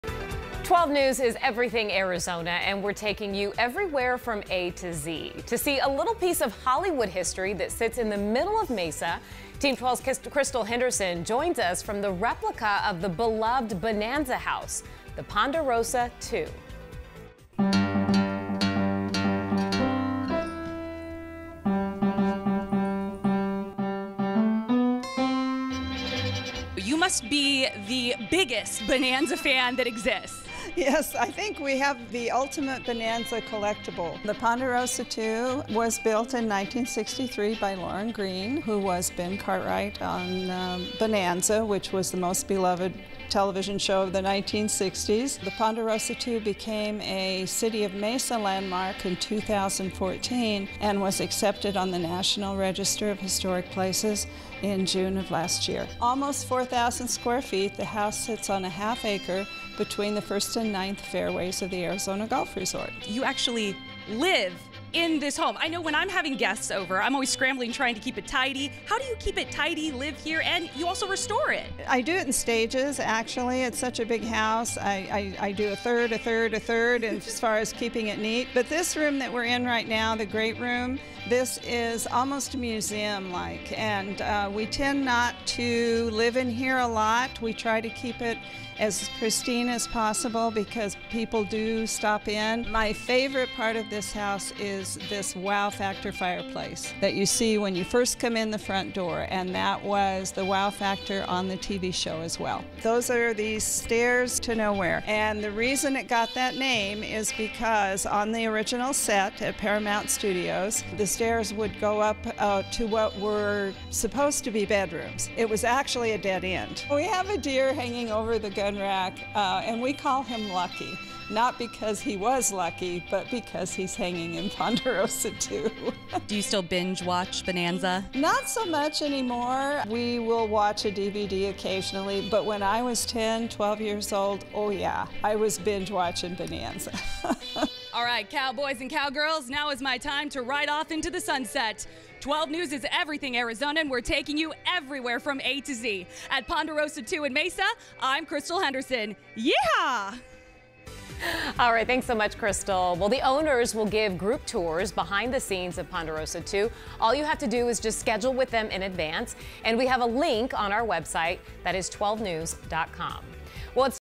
Everywhere A to Z: Hollywood history sits in Mesa
The Ponderosa II in Mesa is an exact replica of the house in "Bonanza". Built in 1963, you can take a tour of the home, but just know it serves as an actual home to Tom and Louise Swann.